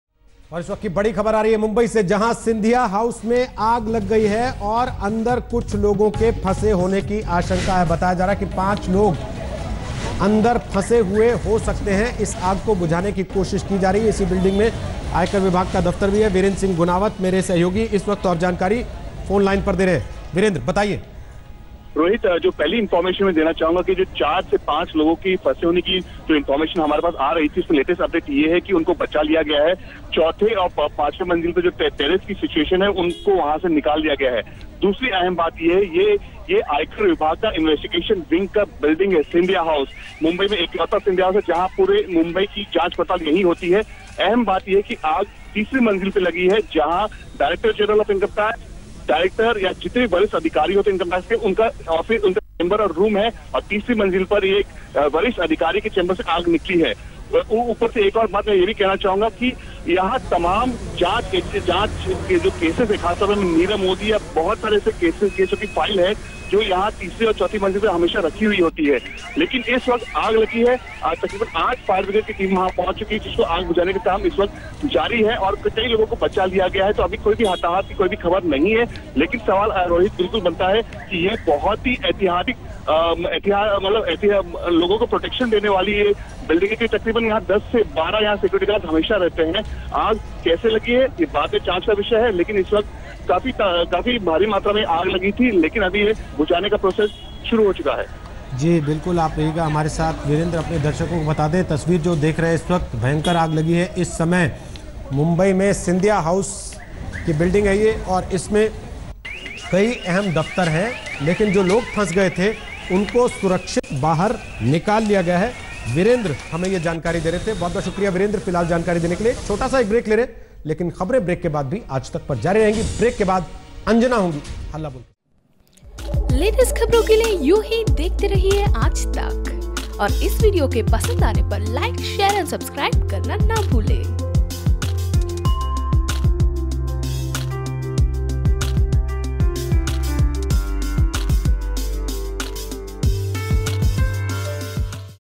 मुंबई के सिंधिया हाउस में लगी आग, वरिष्ठ अधिकारी के चैम्बर से निकली आग | Breaking News
मुंबई के सिंधिया हाउस में  लगी आग, सभी लोग सुरक्षित निकाले गए |